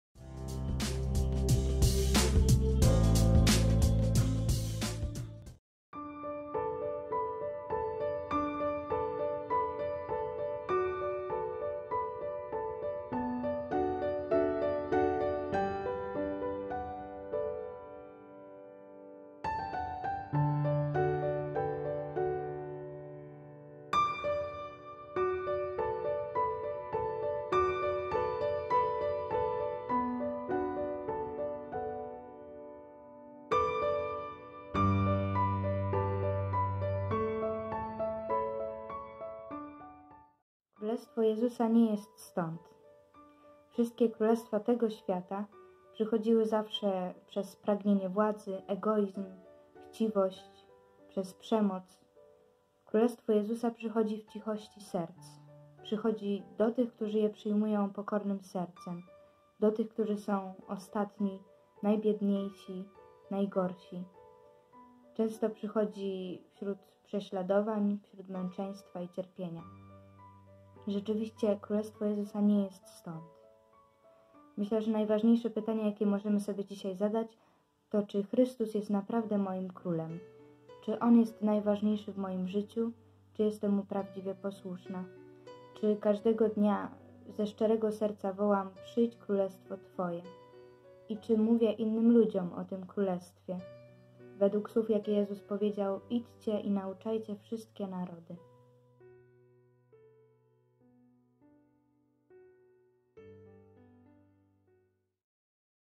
Ewangelia na dziś - 21.11.2021
SERVIAM dla Słowa. Będę służył Słowu.
Siostry urszulanki i młodzież urszulańska rozważa Słowo Boże.
Jest to weekendowe dzielenie się Ewangelią.